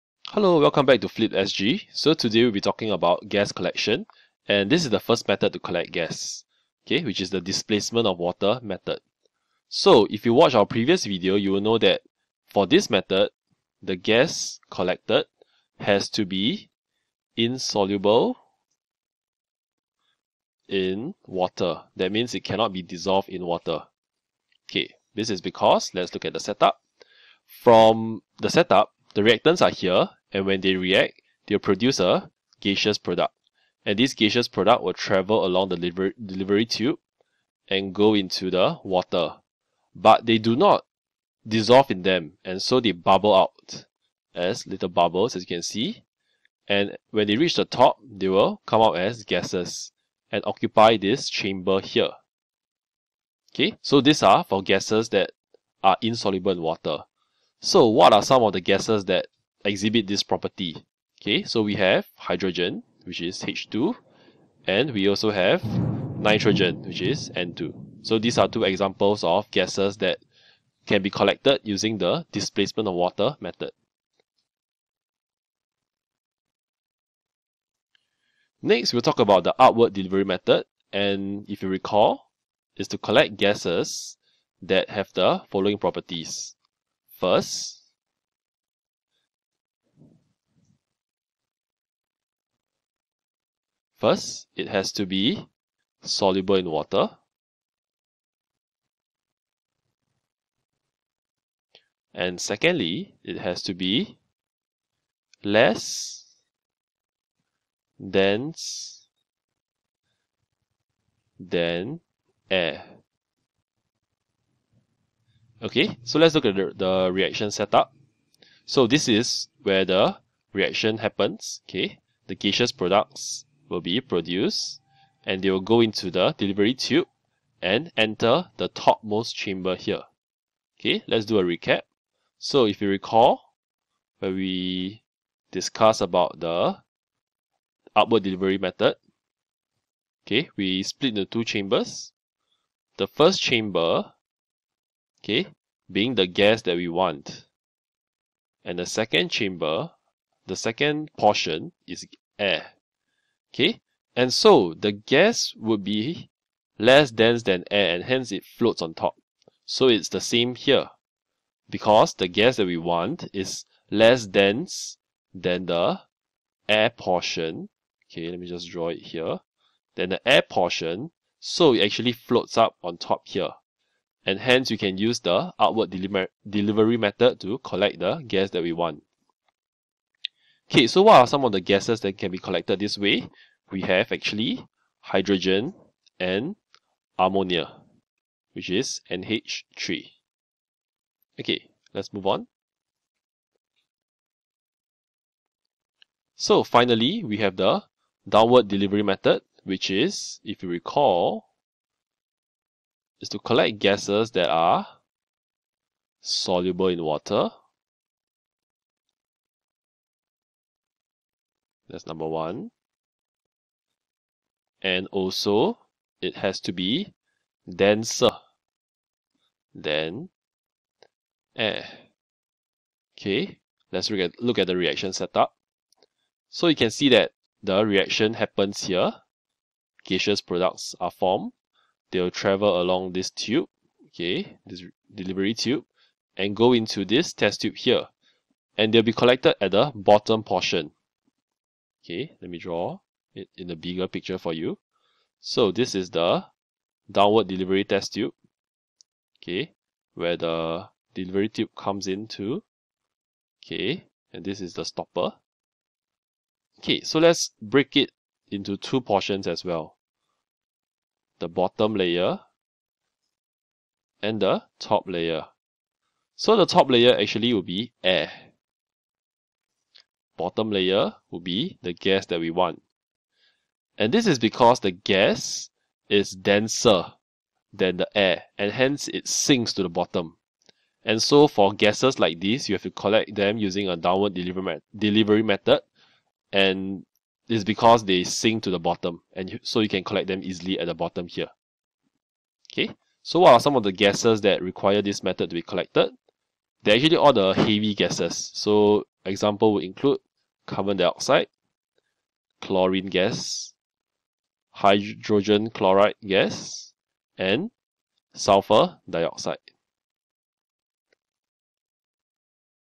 'O' Level Chemistry - Gas Collection Techniques
In this video, we discussed gas collection techniques - Displacement of Water, Upward Delivery and Downward Delivery.